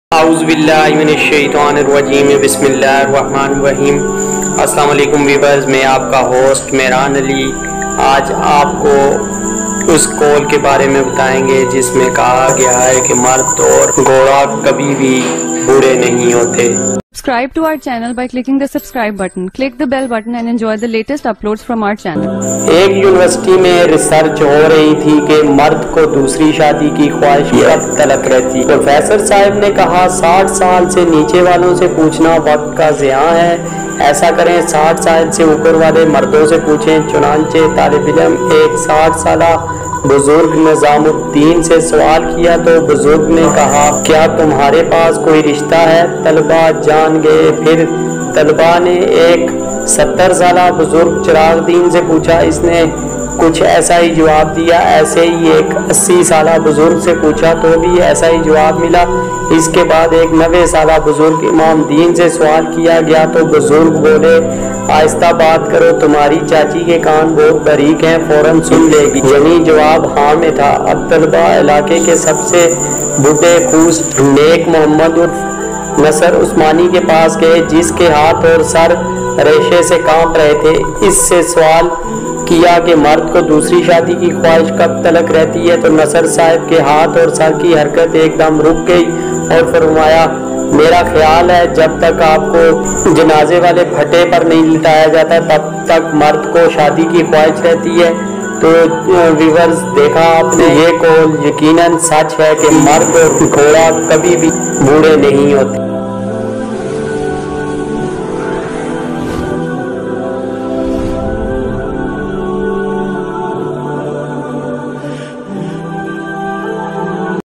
Mard Ko dosri Shadi ki Kawaish kab tak Rati ha !! Mard Or Gorha kabi b boray ni hotay !! دوسری شادی
مرد اور دوسری شادی کی خواہش !! مرد اور گھوڑا کبھی بھی بوڑھا نہیں ہوتا !!